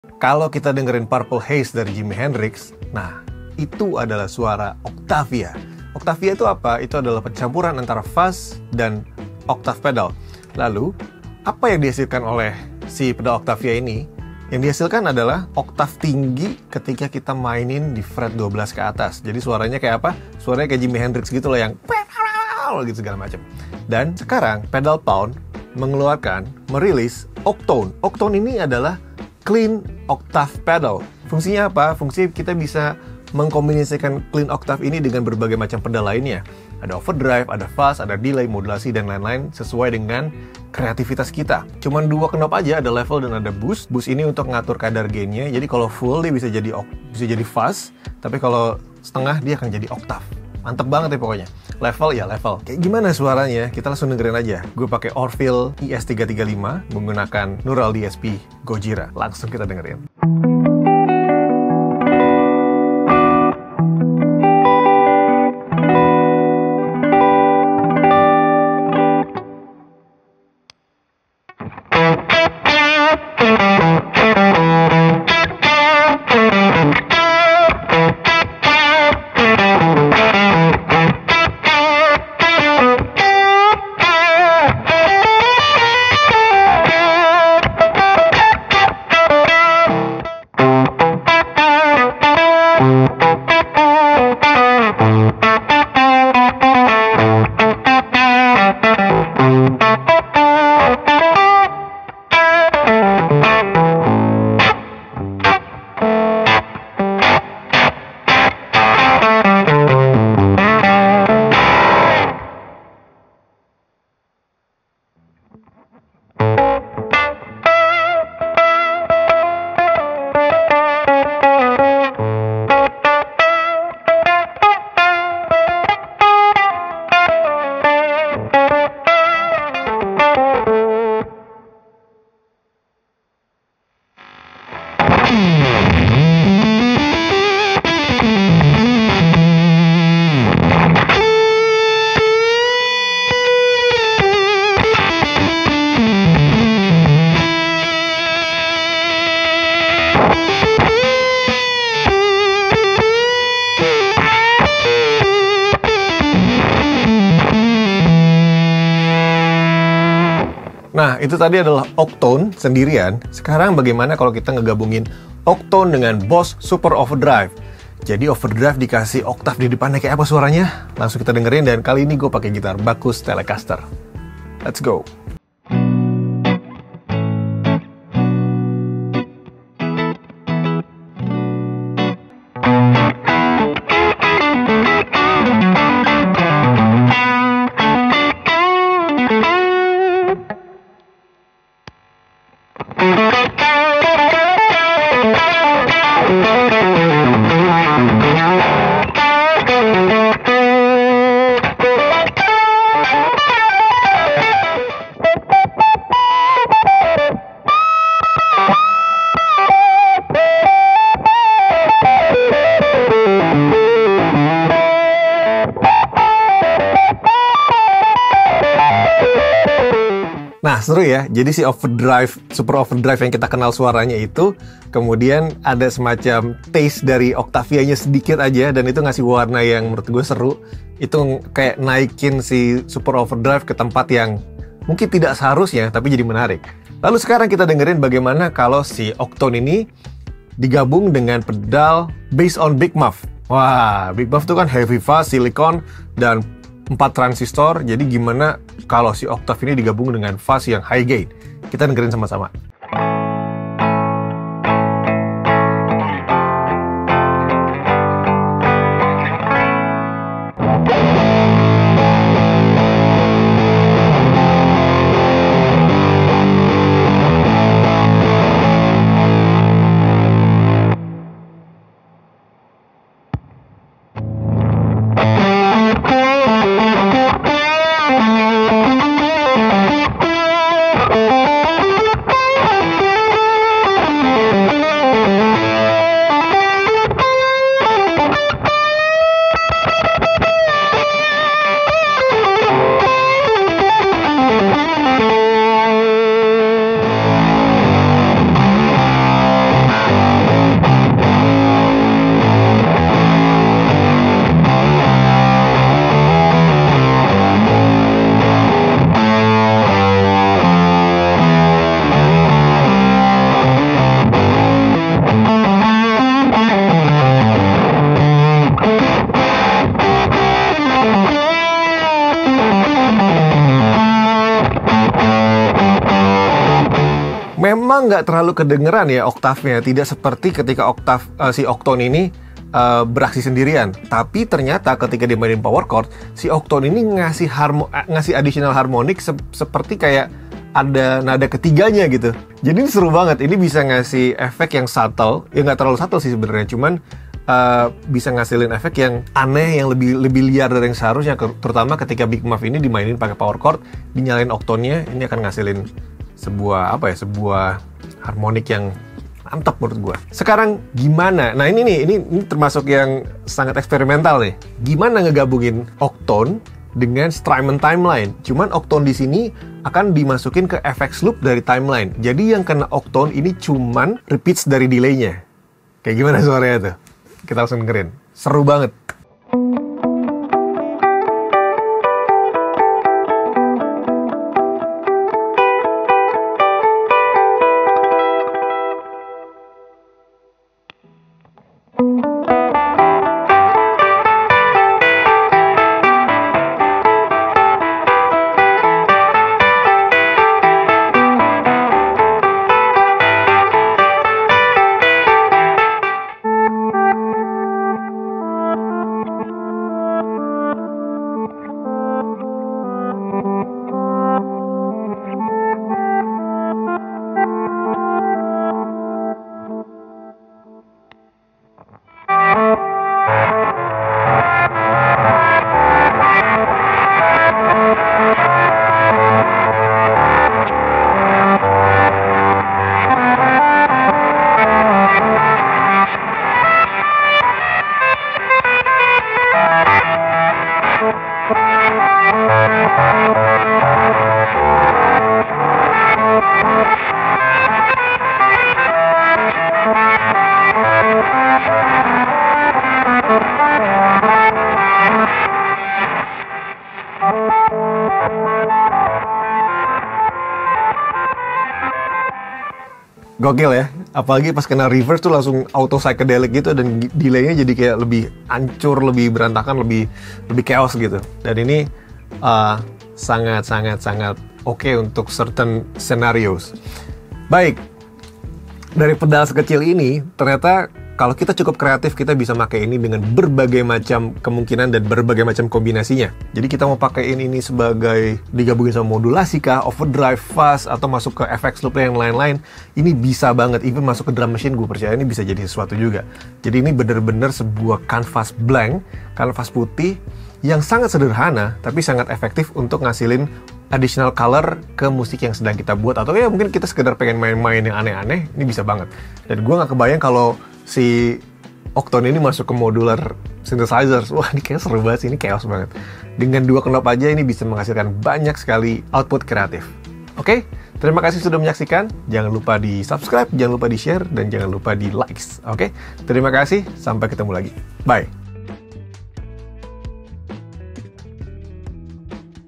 GIMANA CARA MAKENYA?
Pedal octave seru nih

00:00 explanation in Indonesian
01:00 humbucker
02:10 full on fuzz
02:55 octone + SD1
04:12 octone + muff
06:19 experimental!
07:43 petuah Pak RT

Gear used :
Orville ES335
Bacchus T Style
Neural DSP Gojira
Two Notes Cabsim
UAD Apollo 8 Audio Interface and Plugins